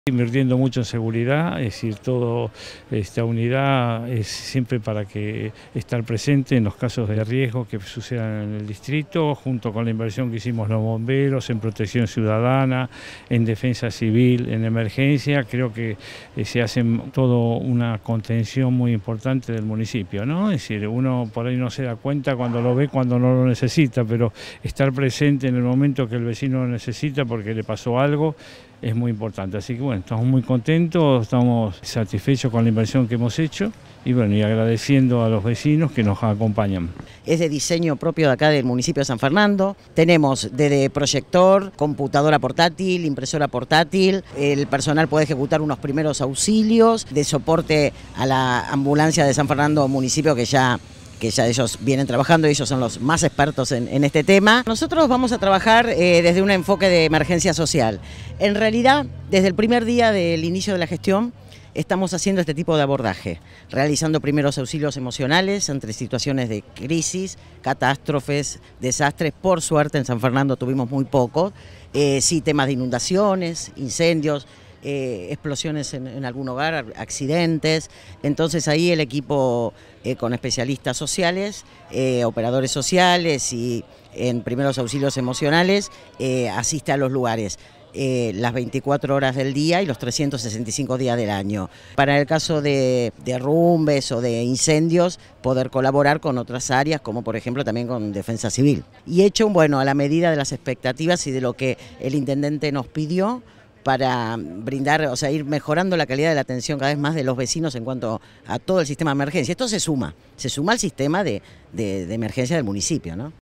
Se sumó un móvil de Gestión de Riesgo al equipo de emergencia municipal
El intendente de San Fernando presentó una nueva unidad de emergencias especializada en gestión de riesgo. El mismo cuenta con un equipo administrativo, de prevención de salud y protección civil.